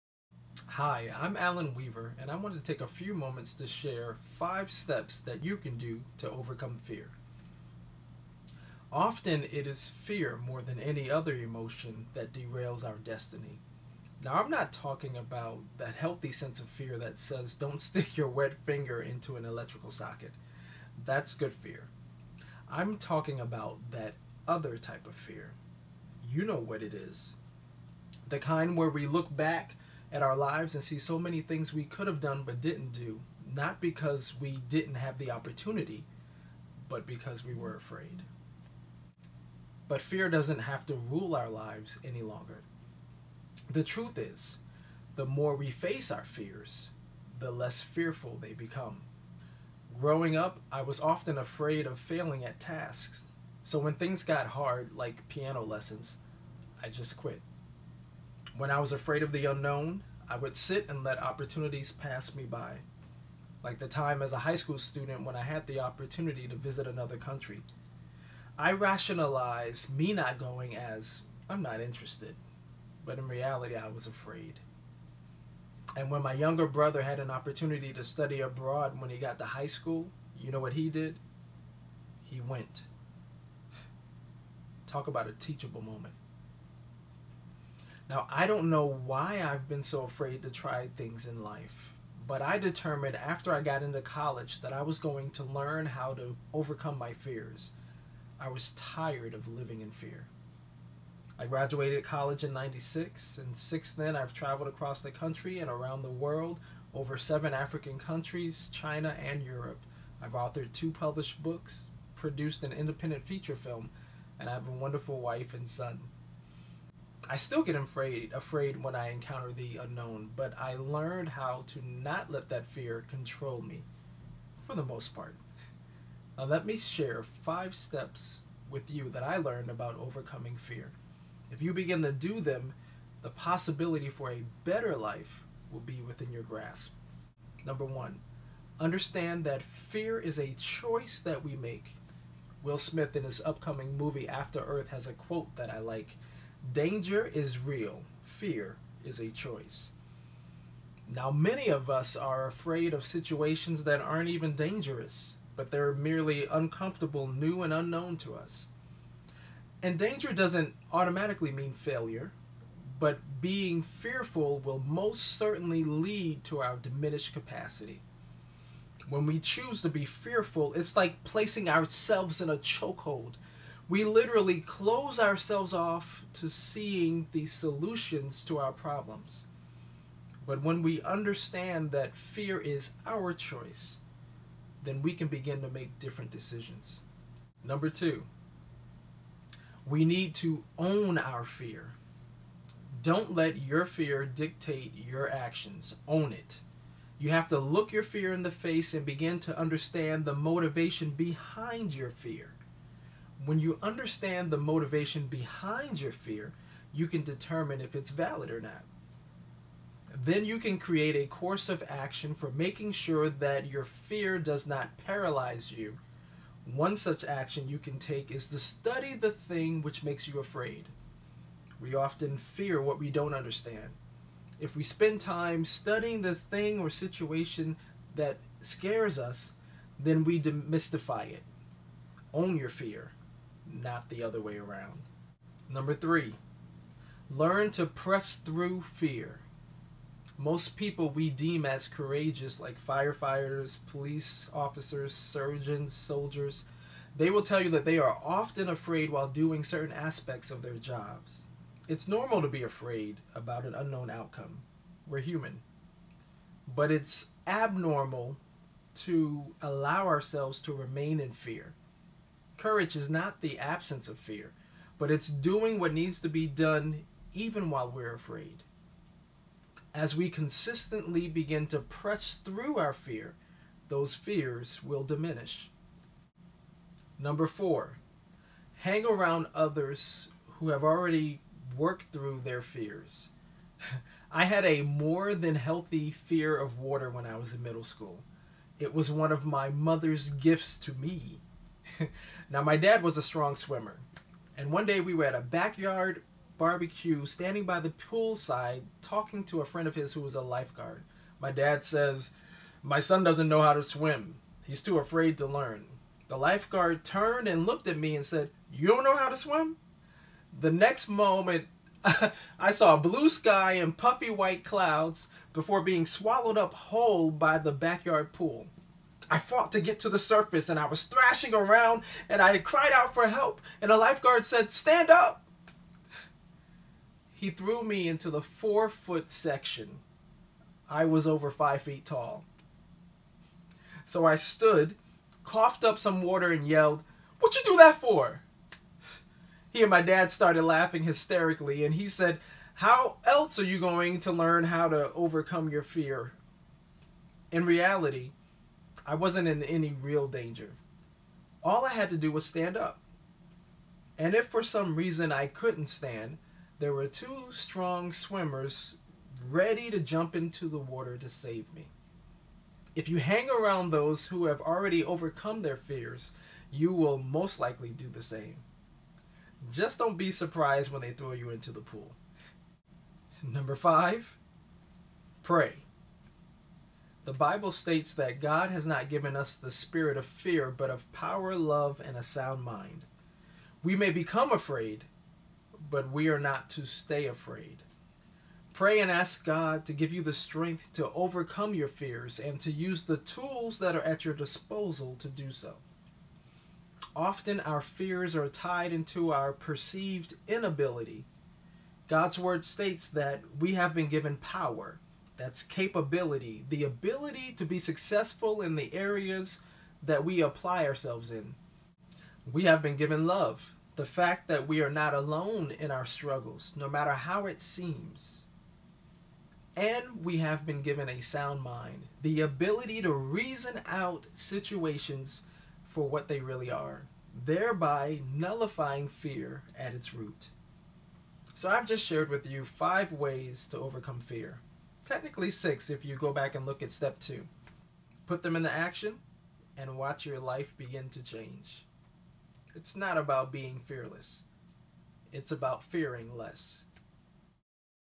5 Steps to Overcome Fear
1/16/13.  Brendon Burchard issued the challenge on his website to create a short video providing "5 Steps to do anything." "5 Steps to Overcome Fear" is my response to that challenge.

In this video, I include some of my own personal struggles and victories pertaining to "fear" while sharing 5 Steps viewers can use to help overcome fear in their own lives.